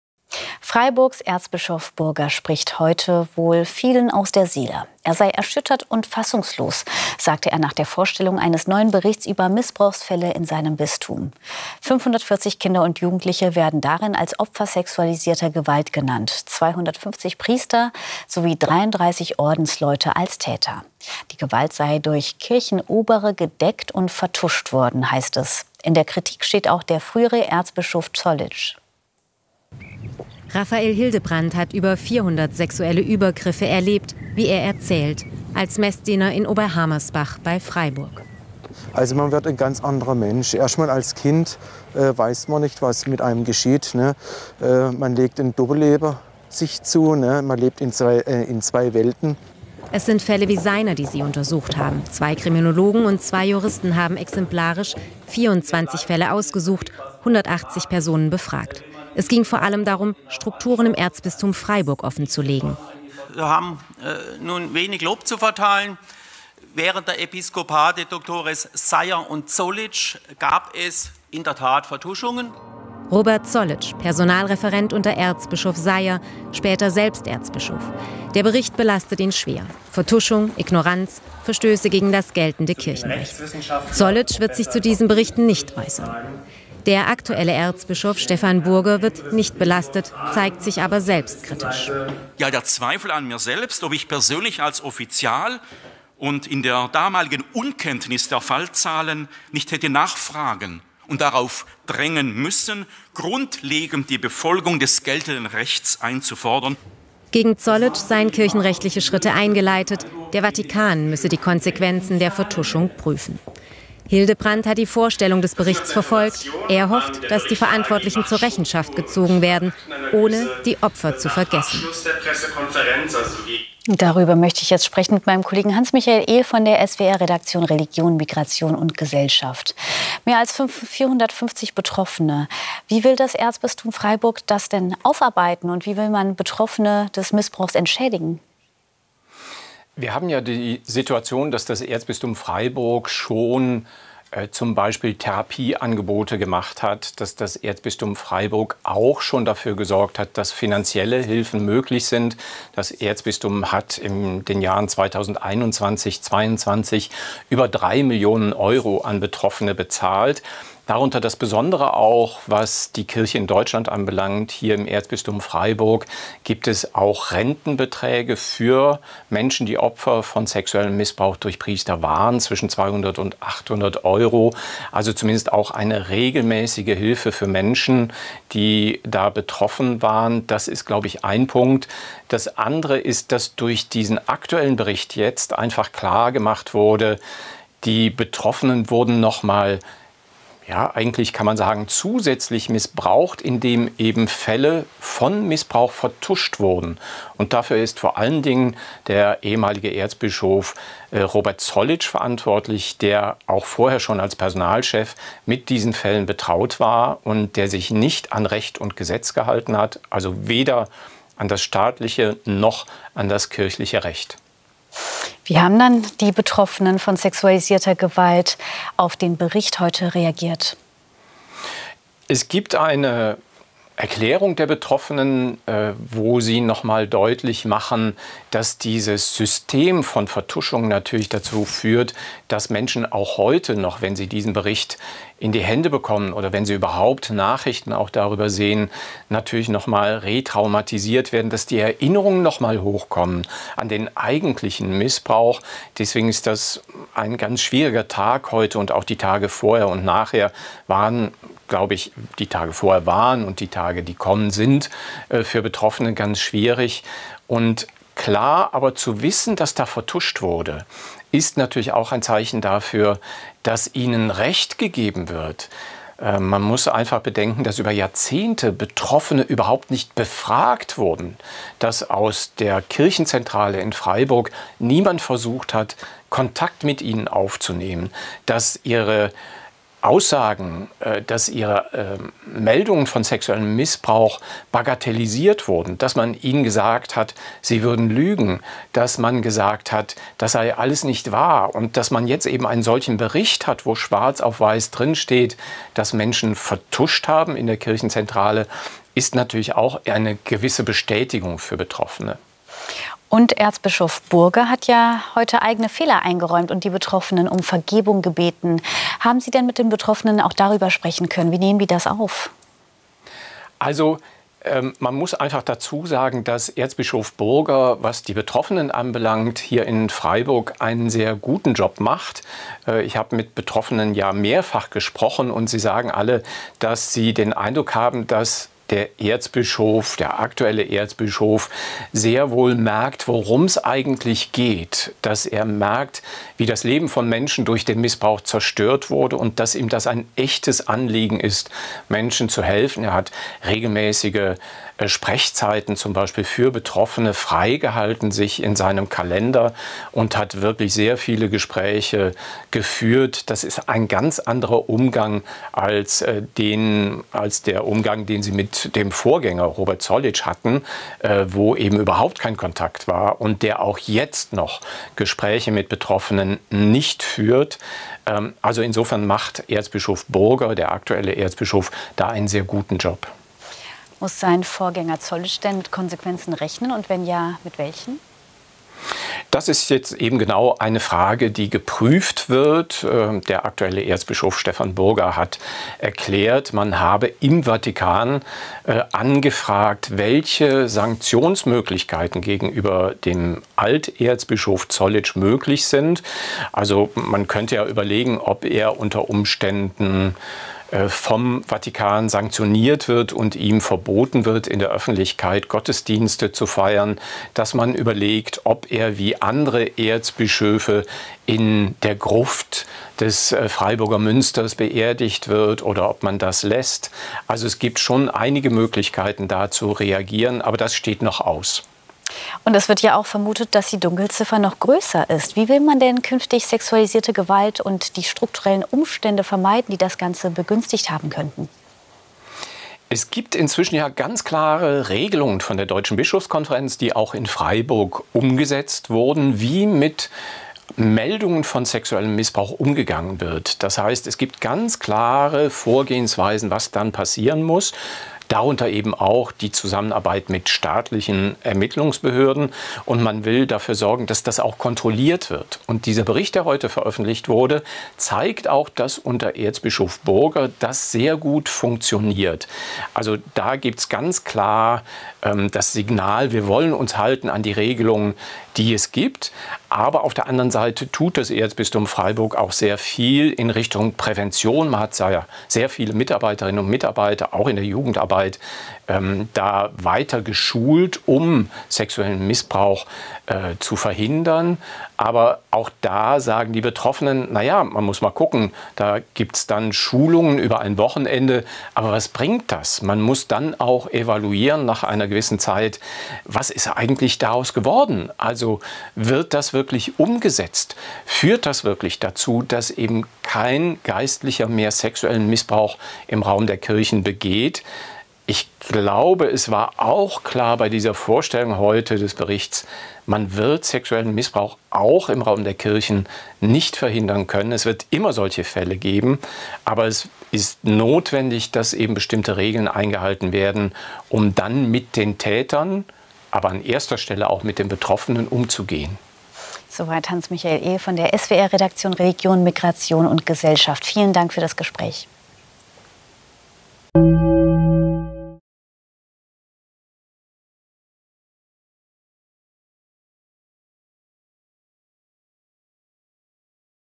Missbrauch im Erzbistum Freiburg: Mehr als 250 Beschuldigte Kleriker
Im Erzbistum Freiburg sind mehr Menschen von sexualisierter Gewalt durch Geistliche betroffen als bisher offiziell bekannt war. Es werde nun von mehr als 540 Betroffenen ausgegangen, sagte der Vorsitzende der Aufarbeitungskommission, Magnus Striet. Zudem gebe es mehr als 250 beschuldigte Kleriker. Die Dunkelziffer sei vermutlich deutlich größer.